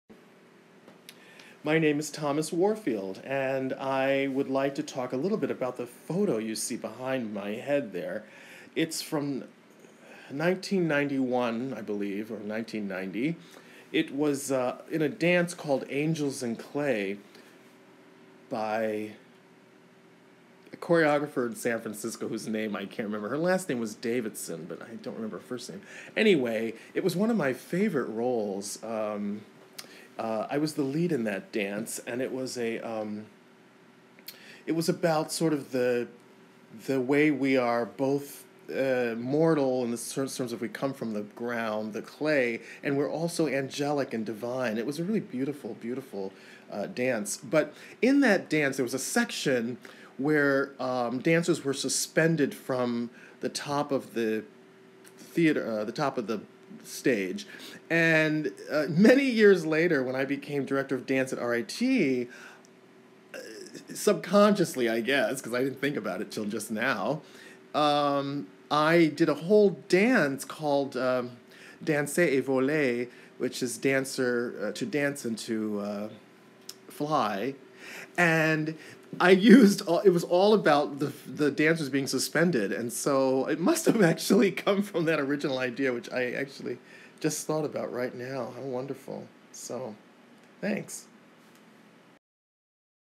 Thomas Warfield
Connecting to past through Dance
Remembering the common theme of flight in both “Angels in Clay” and “Danser et Voler” March 29, 2019. Thomas explains his lead in the dance.